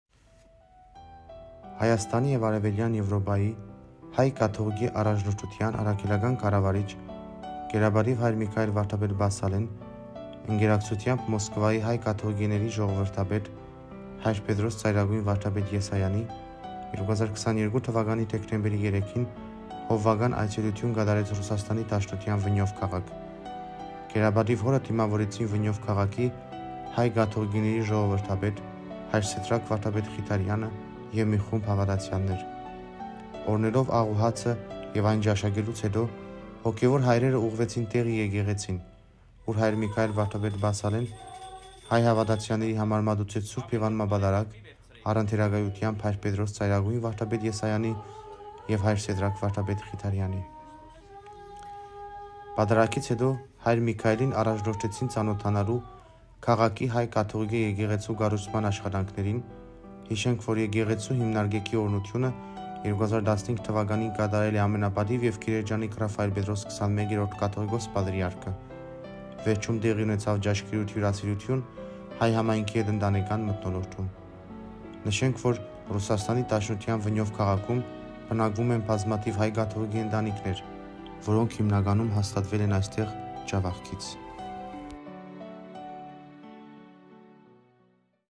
Գերապատիւ Հայր Միքայէլ Վրդ. Պասսալէի հովուական այցելութիւնը Վենյովի հայ կաթողիկէներուն։
Հայաստանի եւ Արեւելեան Եւրոպայի Հայ Կաթողիկէ Առաջնորդութեան  Առաքելական Կառավարչ՝ Գերապատիւ Հայր Միքայէլ Վրդ. Պասսալէի հովուական այցելութիւնը Վենյովի հայ կաթողիկէներուն։